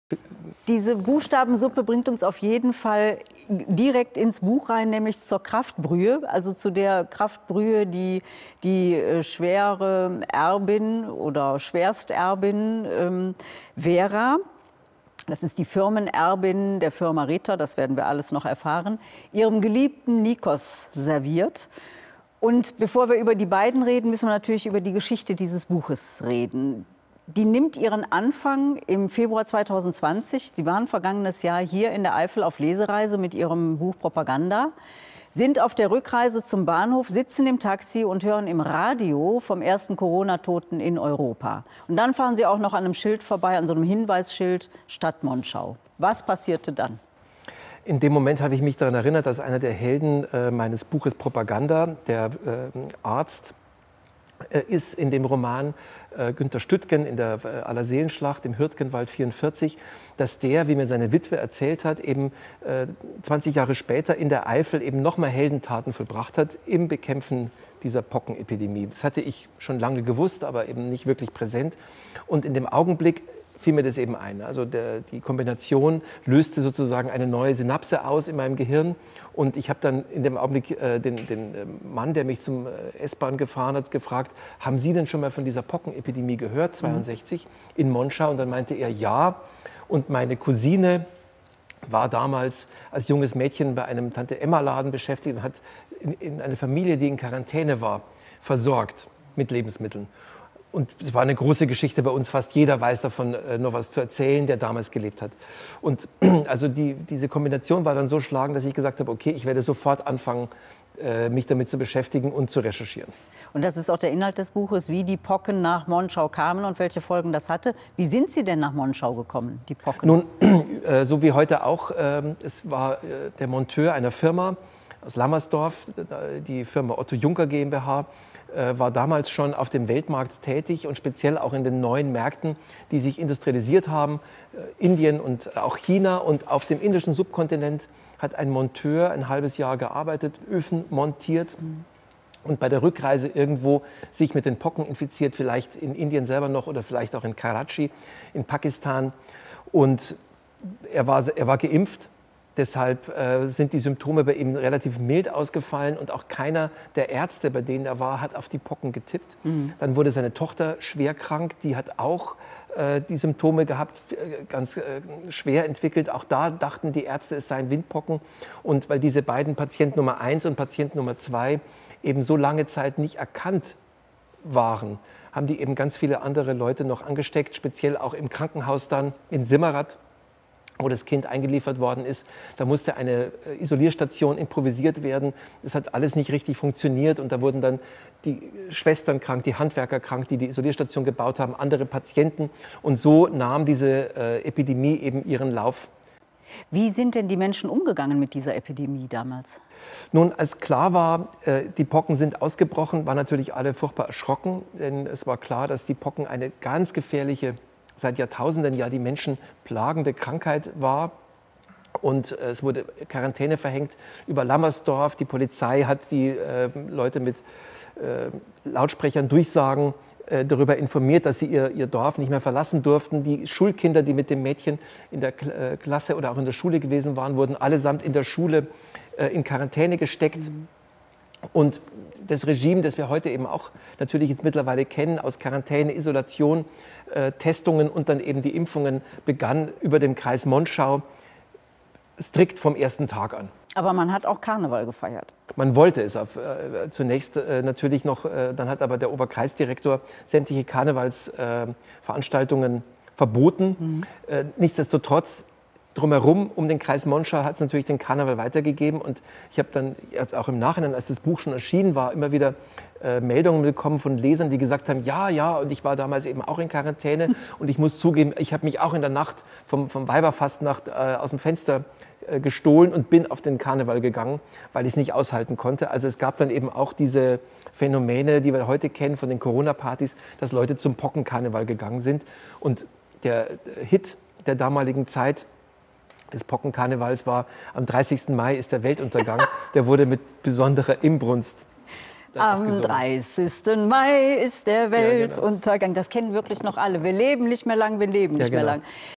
EifelDreiTV #!AusLese2
Unser Gast, der Bestellerautor Steffen Kopetzky erzählt im Gespräch mit Gisela Steinhauer von der Entstehung des Romans Monschau.